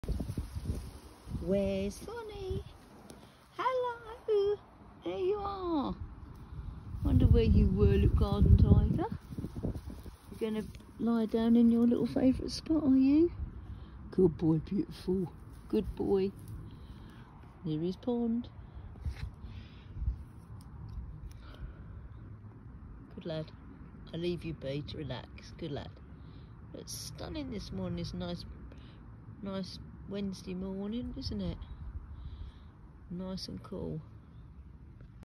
WHERE’S THORNY? cathunt this morn 2 find where the Tabby is ?
Thorny is found out the front garden this morning 😻